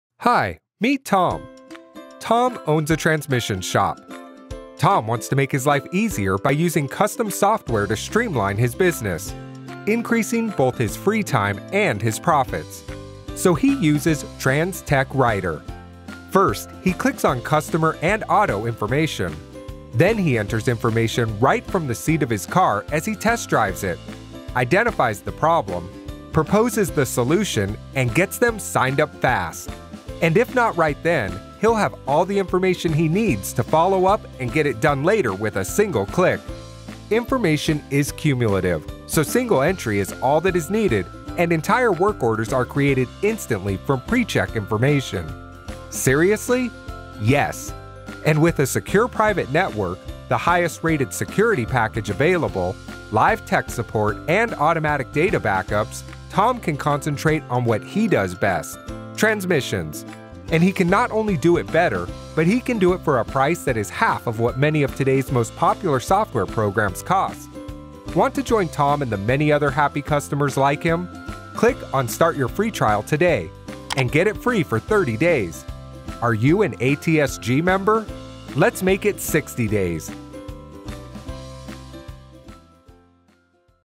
Trans Tech Writer: How Our Transmission Shop Management Software Works
Trans Tech Writer is a state of the art transmissions shop management software that allows the shop owner to customize the shops daily activity to the software and not the other way around.

If you are an ATSG - Automatic Transmission Service Group Member you get 60 days FREE.
If you are not an ATSG Member, you can sign up